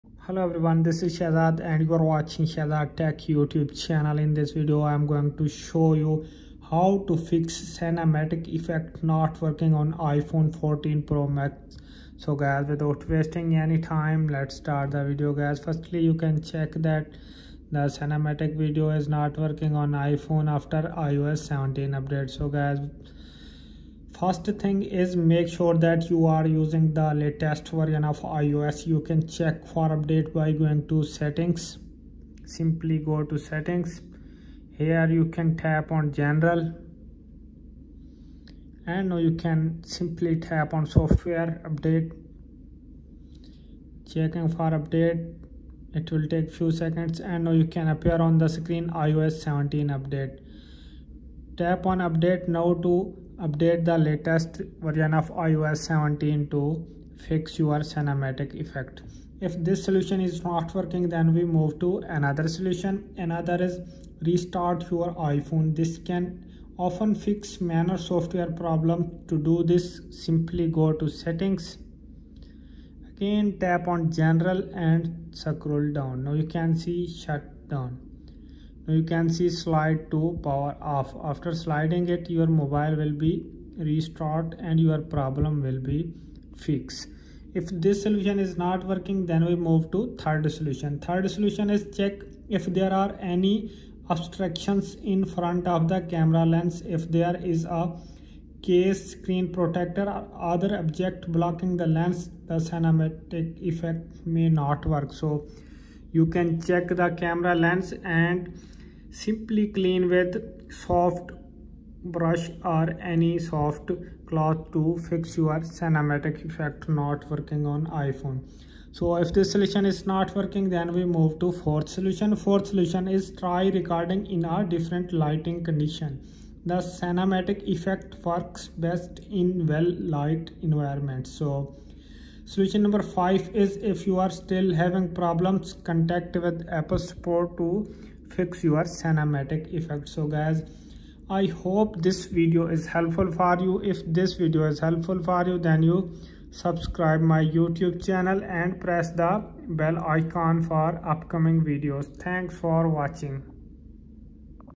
How To Fix Cinematic Effect Not Working On iPhone 14 Pro Max After iOS 17 Update|Fix Cinematic issue
After Watching This Video We Learn how to fix How To Fix Cinematic Effect Not Working On iPhone 14 Pro Max After iOS 17 Update|Fix Cinematic issue